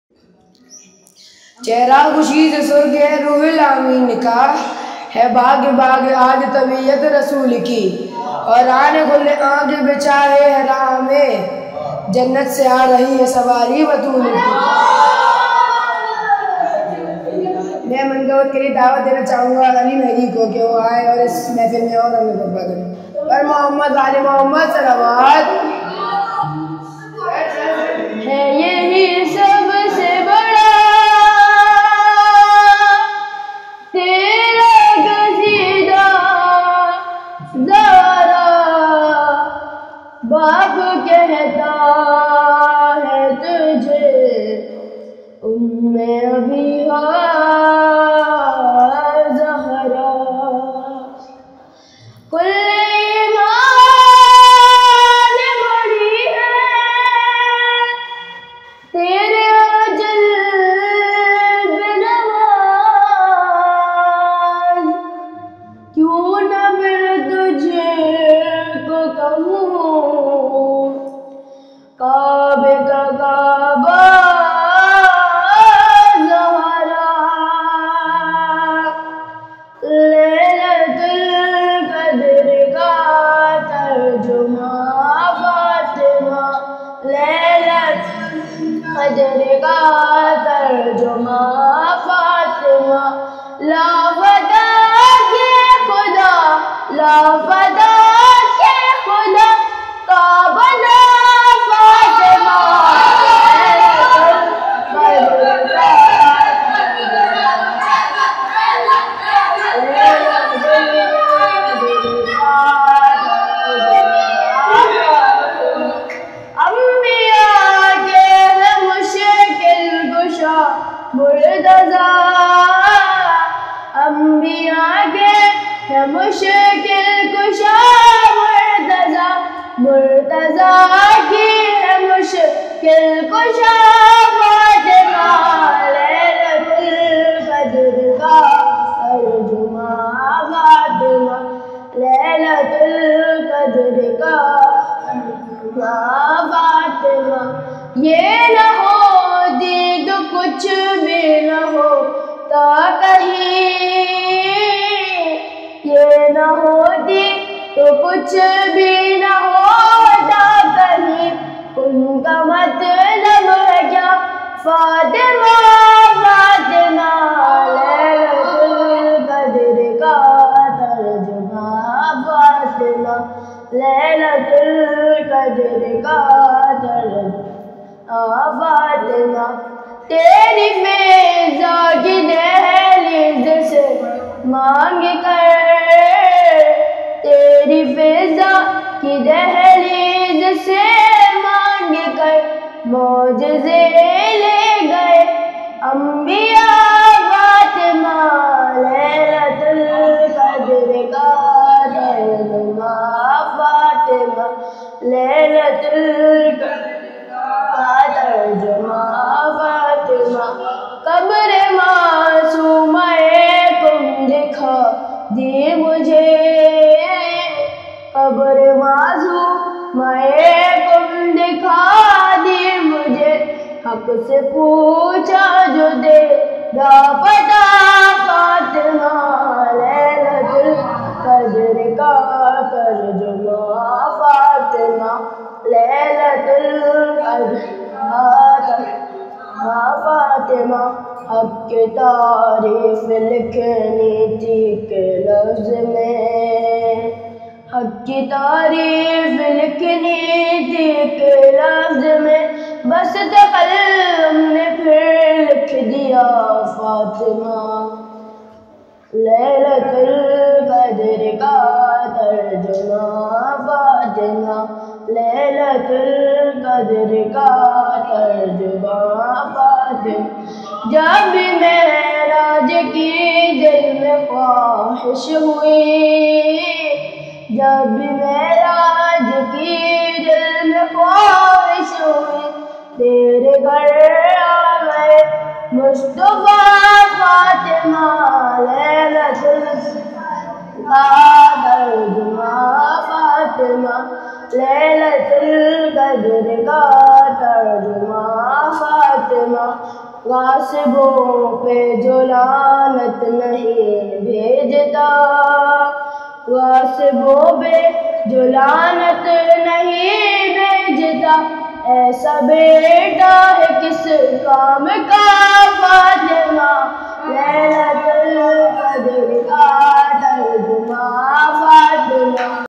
Lailat ul Qadar Ka Tarjuma Hai Fatima | Sunday Youth Class | Jashan Wiladat e Zehraس | Aabtaab
Share This Channel With Your Friends And Family